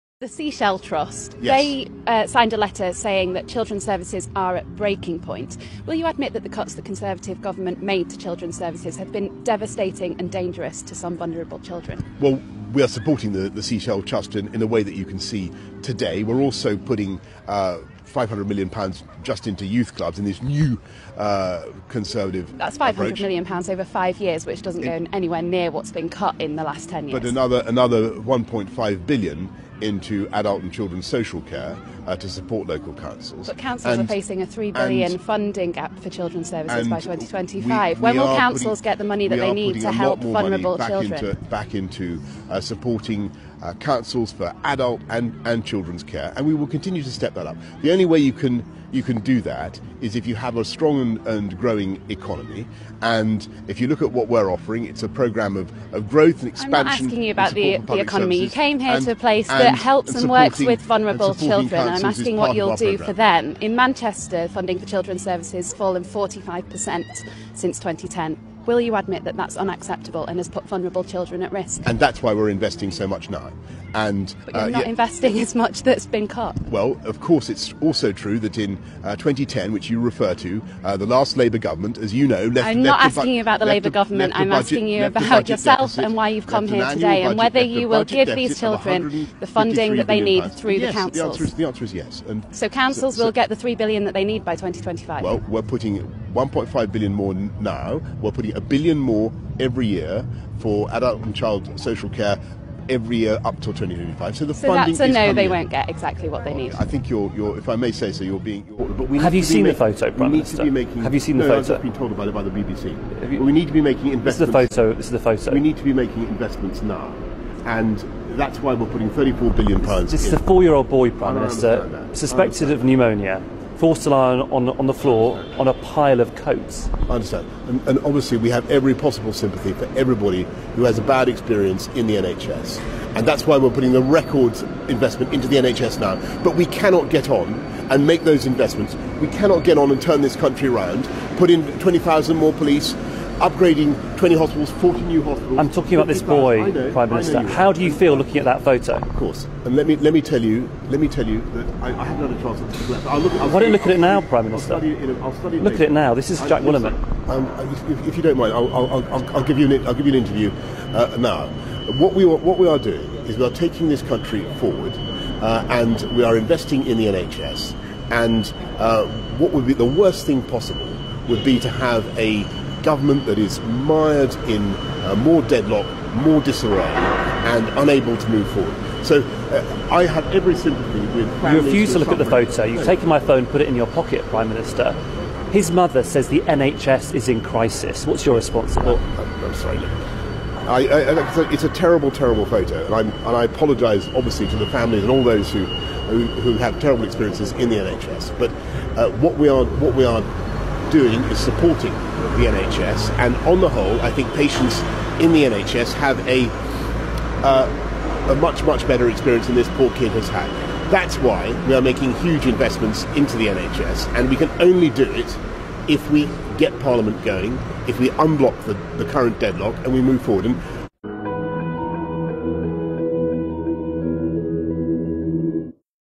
At `breaking point' Mr Johnson
***Johnson confronted about boy at Leeds hospital is halfway through*** Two regional journalists get the better of PM Johnson here, simply by holding him to account on his government’s record. There’s a canny strategy for you big time National reporters (Peston/Kuenssberg etc) to consider borrowing yourselves.
The first holds him to account brilliantly on his government’s failure to spend on children’s services - despite the PM attending a PR event in the hopes it would make him look like he gave a crap. The answers he gave under duress are not mirrored in his party’s manifesto, so the money he promises is unlikely to exist.
The second journalist shows the PM the picture we shared here yesterday of a 4yo boy with Pneumonia resting on coats, waiting for 13 hours for a bed a Leeds General Infirmary. He asks the PM to look at the picture. The PM instead, hides the journalists phone in his own pocket off camera! Until the journalist calls him out on it! Then the PM acts coy, and pulls the phone out of his pocket, gives a half hearted apology to the boy and bumbles some nonsense about Labour.